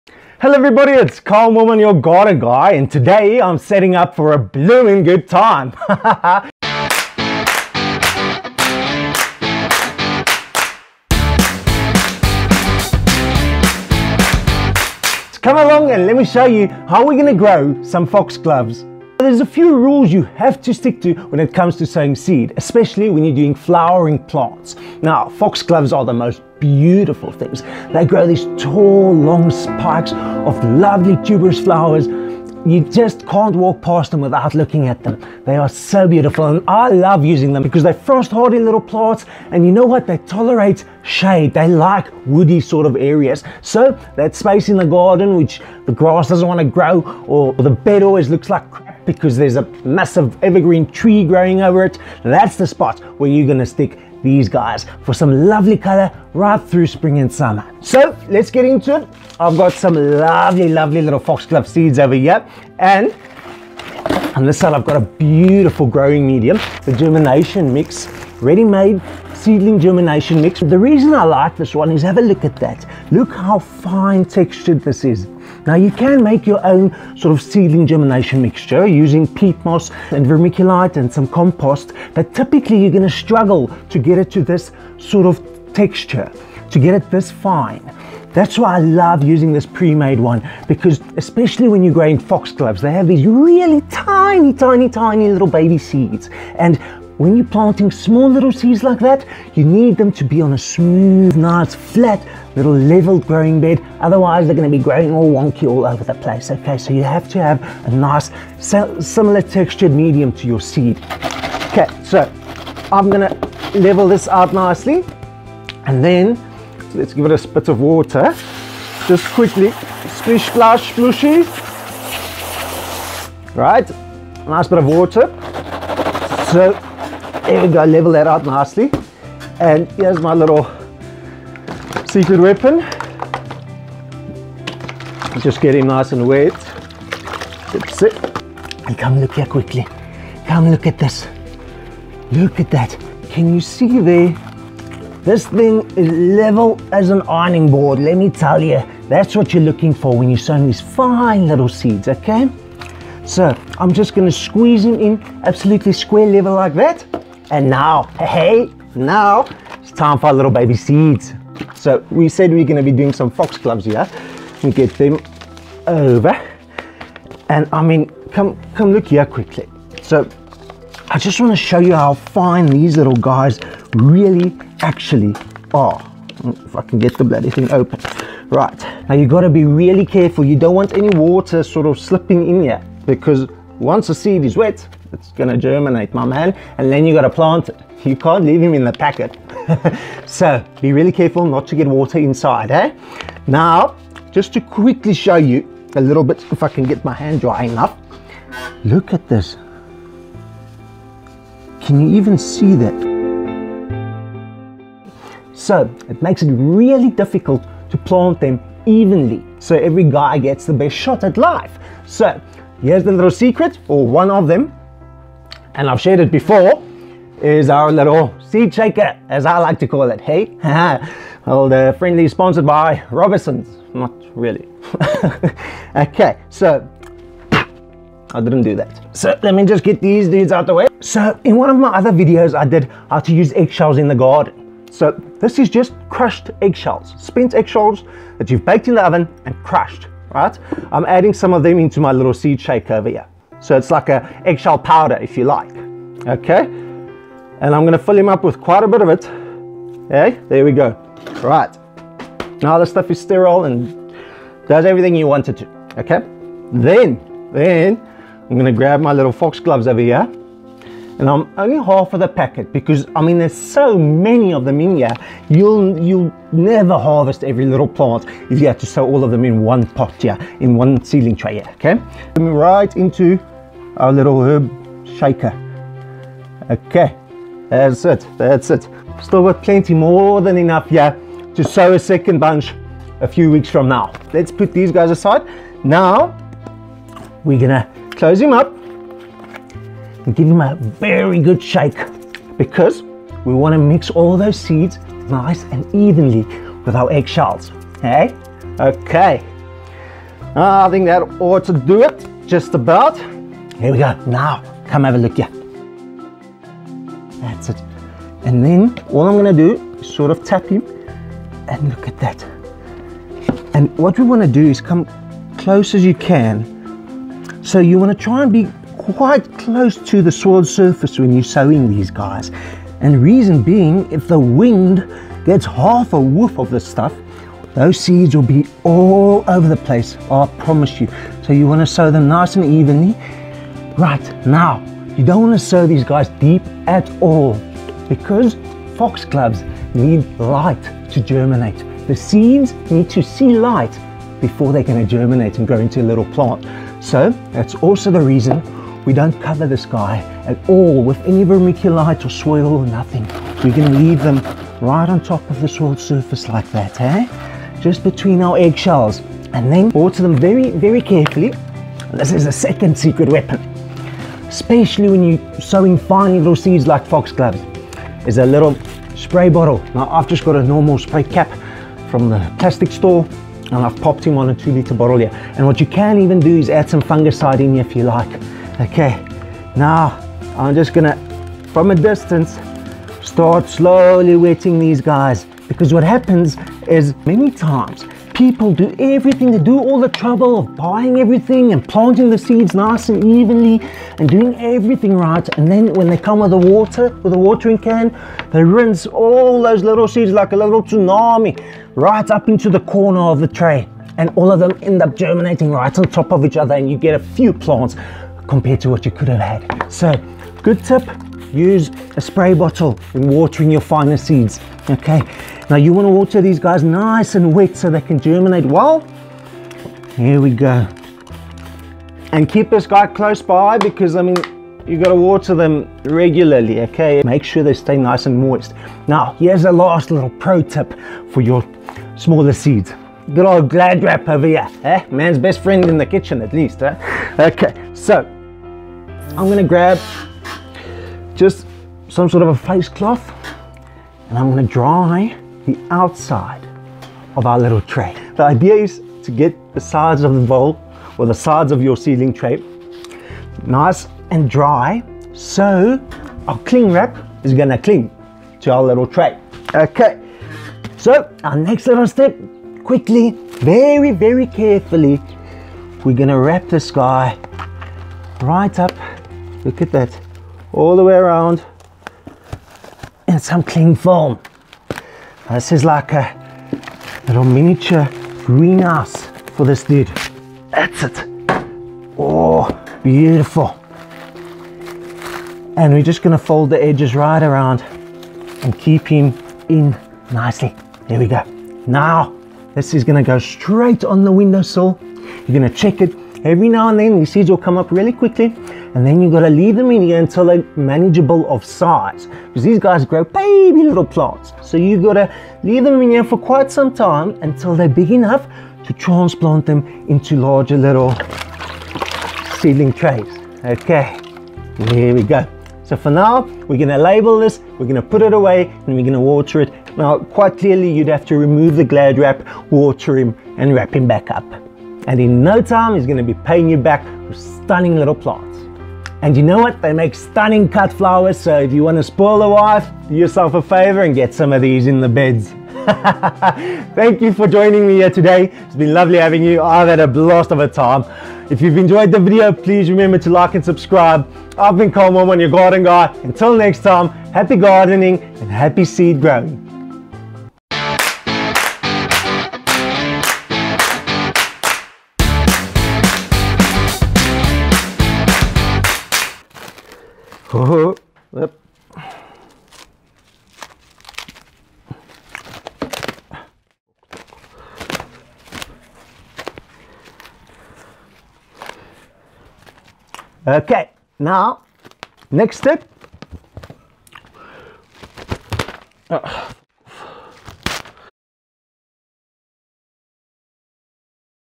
How to grow foxgloves from seed. How to plant foxgloves aka Digitalis purpurea in the garden.
How to sow foxglove seeds, garden guy explauns when to plant foxgloves in the garden and shows you how to grow Digitalis purpurea. What conditions deo foxgloves prefer and where do foxgloves like to grow.